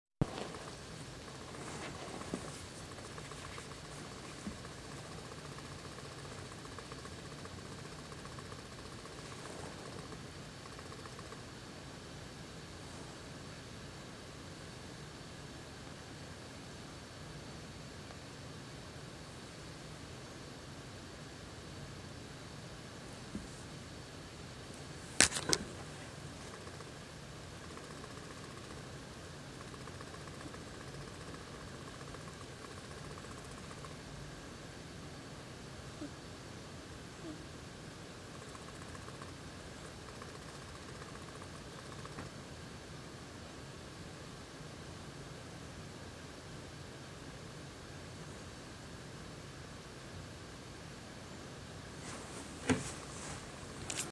Rochelle Eye Checkup 11-02-2016
Just checking on Rochelle's "down" eye, which needs daily lubrication.